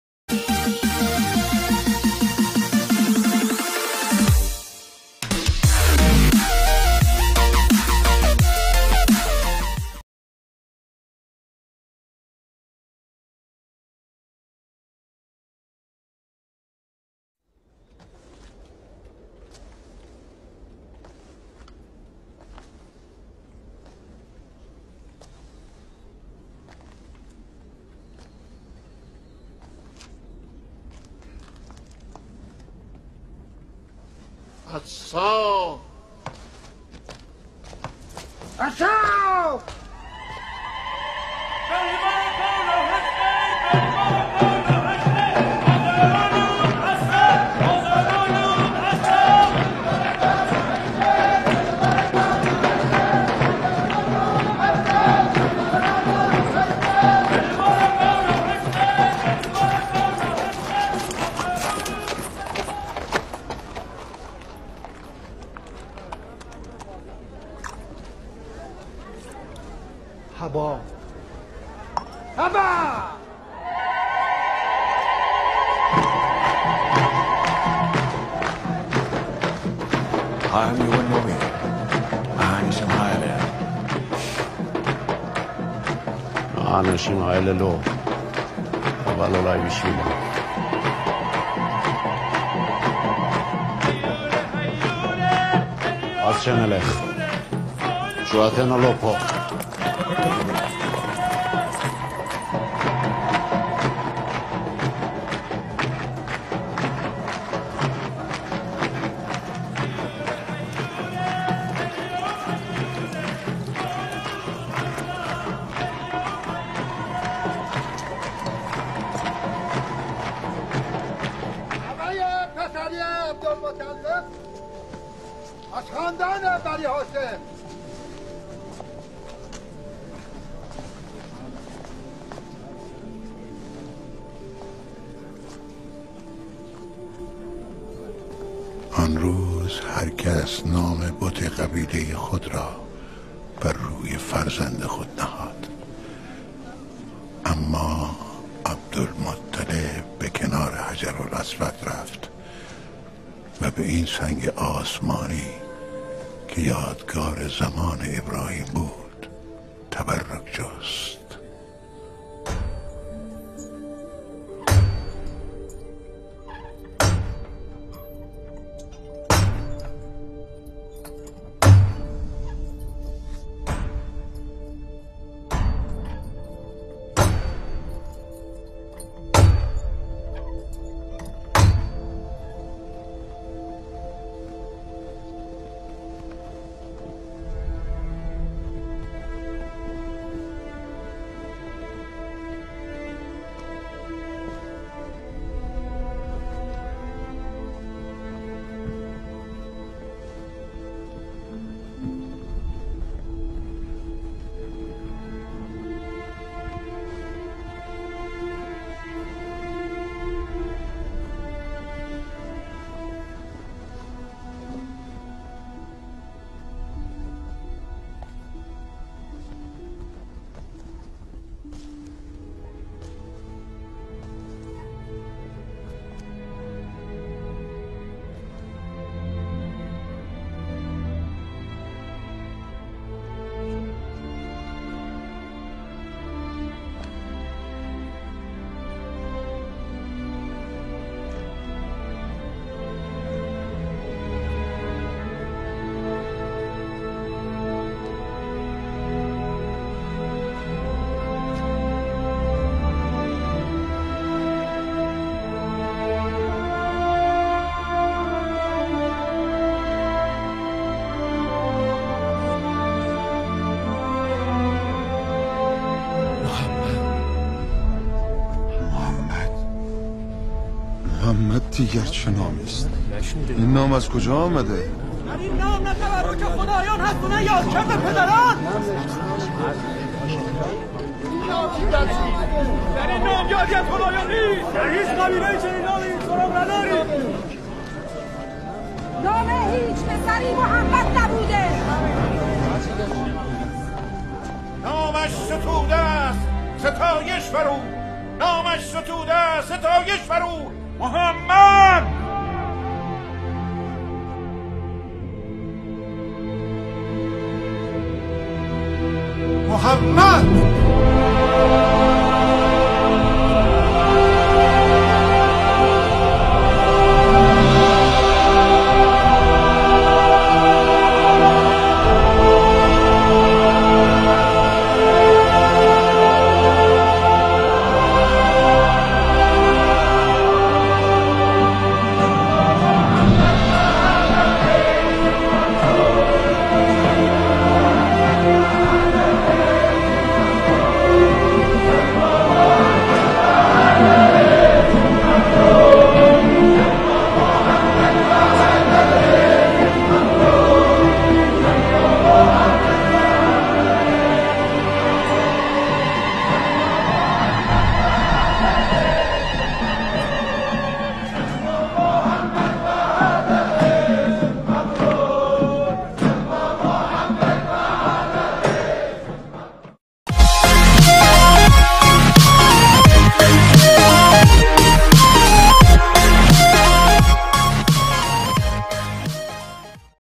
Detik2 Pemberian Nama kepada Baginda Nabi Muhammad SAW ( ilustrasi )
Berikut ini adalah video ilustrasi pemberian nama kepada Nabi Besar Muhammad SAW. Semoga setelah melihat dan menonton video ini, ilmu dan wawasan kita semua makin bertambah. Amiiinnn...